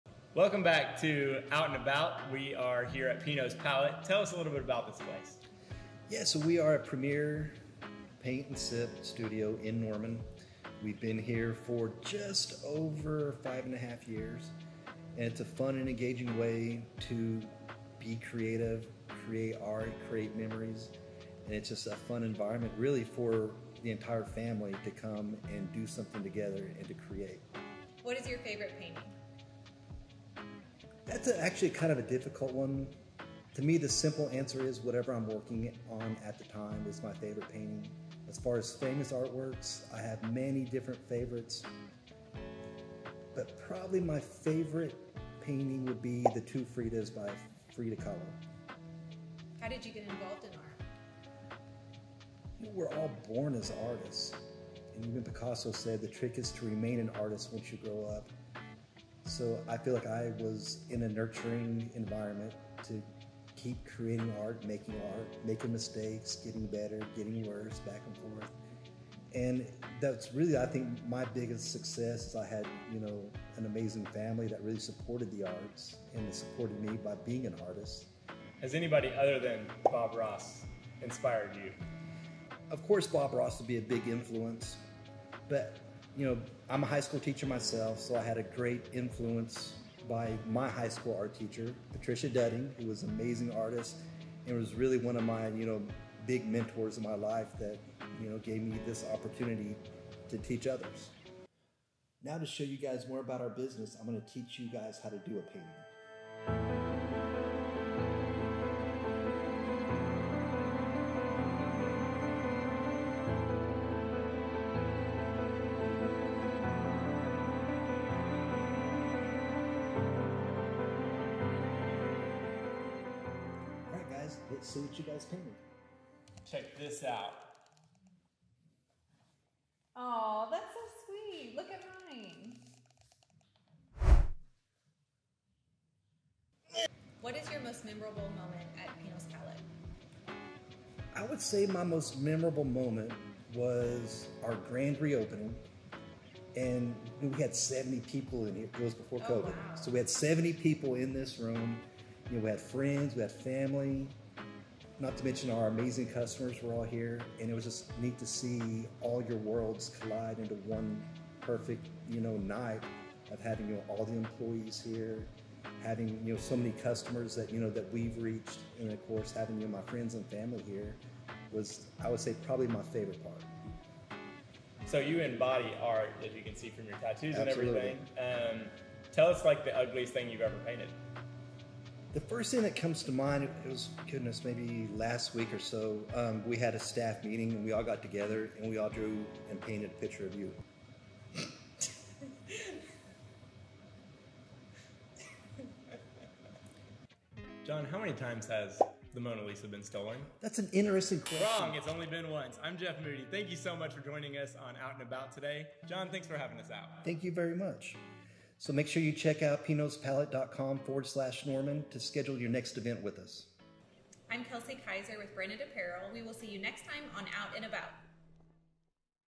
Out & About Pinot's Palette Norman OK!
Paint your heart out at Pinot's Palette here in Downtown Norman Oklahoma. This guided painting experience is perfect for the newbies and experts alike. Sip some wine and and have a good time at Pinot's Palette.